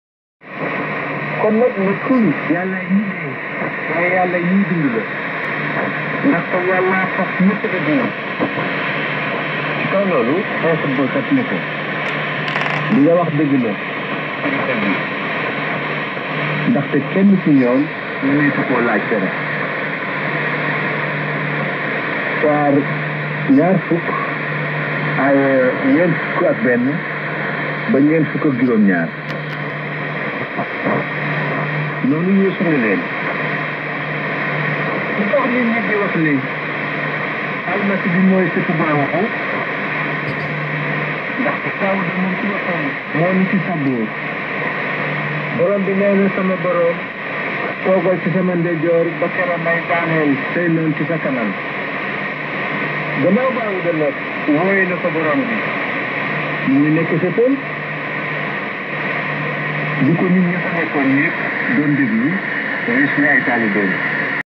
11750 kHz: Adventist World Radio, via Nauen GERMANY
11750 kHz: Adventist World Radio, via Nauen GERMANY, reception in São Bernardo SP BRAZIL - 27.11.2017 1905 UTC. Distance: 10.252 km.
Rx: Icom IC-R75 + RGP Coaxial Cable Square Loop Antenna. Tx w/218º Az, in Wolof language to Western Africa (ITU 46) - Senegal.